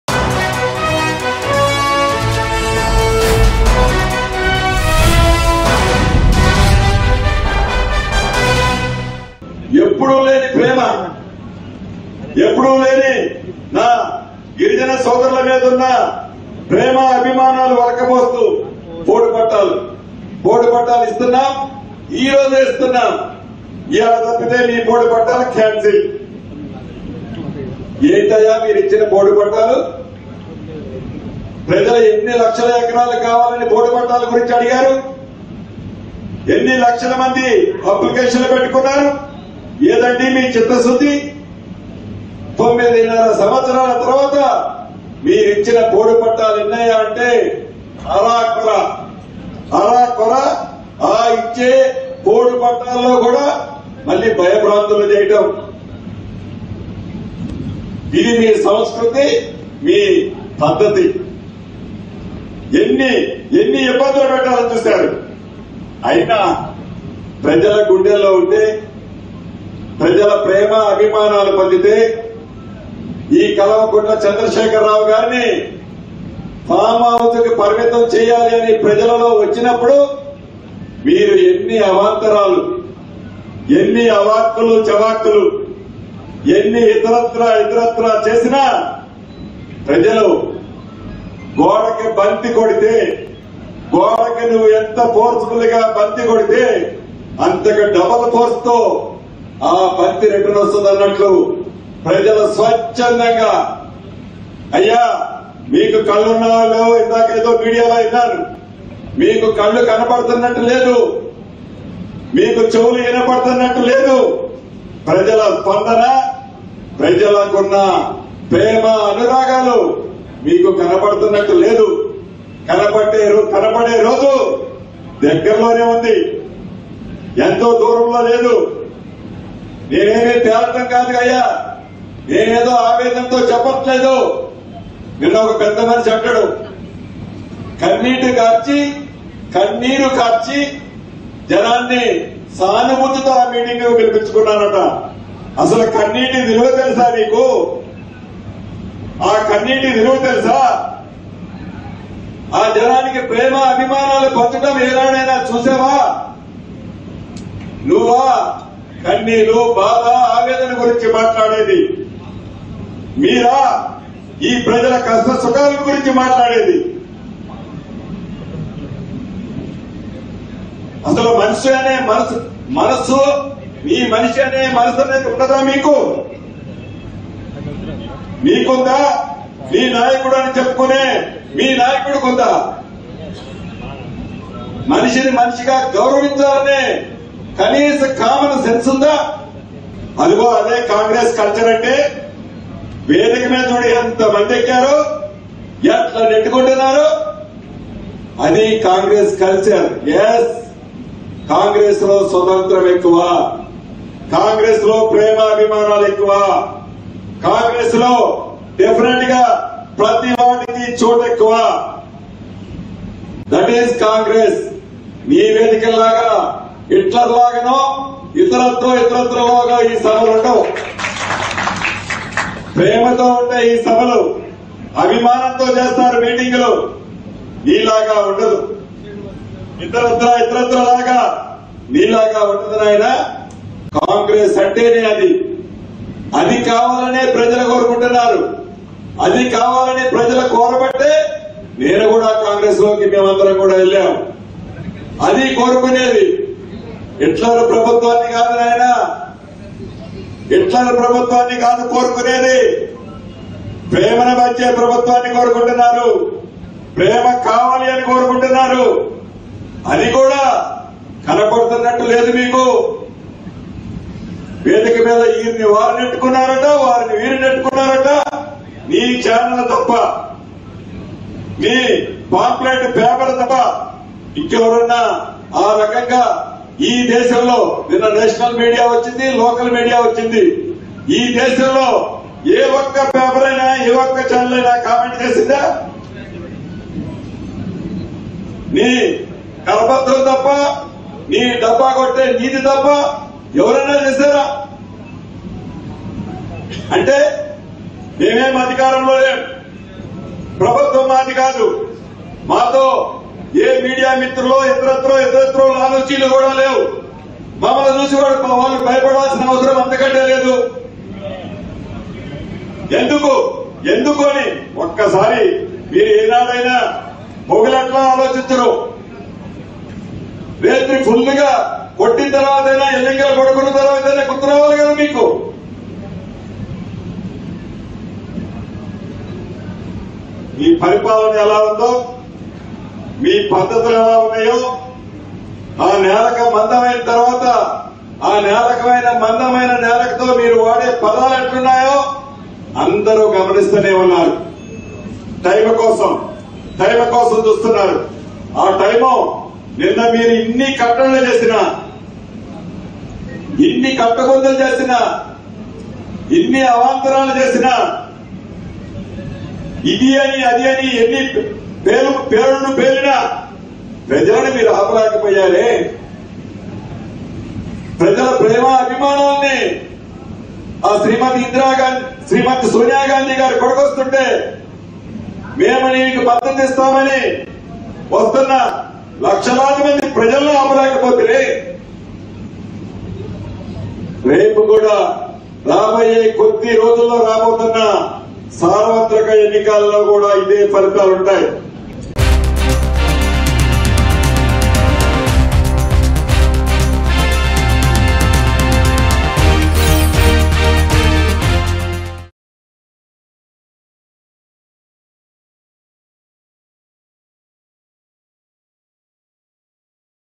Ponguleti Srinivas Reddy Sensational Press Meet After Joining Congress Party | TS News | Mango News
Ponguleti Srinivas Reddy Sensational Press Meet After Joining Congress Party.